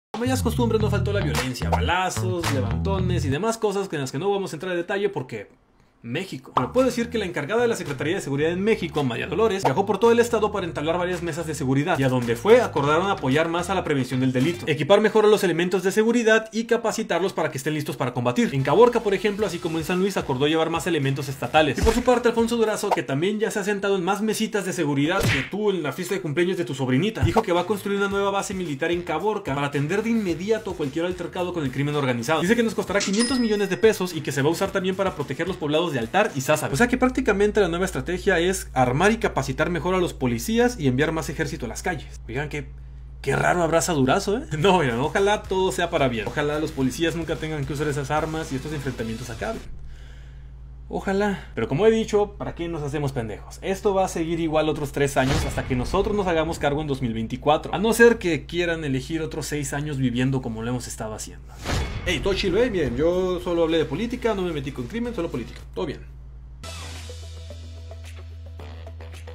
¿Y la paz en Sonora?
Es cierto que la violencia nos ha rebasado, y que nadie puede ahondar en el tema porque... México. (Me mama estar vivo) pero al menos podemos saber que carajos están haciendo las autoridades:

Muchas mesitas de seguridad, aumentar el ejercíto en las calles, capacitar y armar mejor a los policías y construir nuevos cuarteles militares con inversiones millonarias, todo esto, claro, para duplicar los abrazos 🥰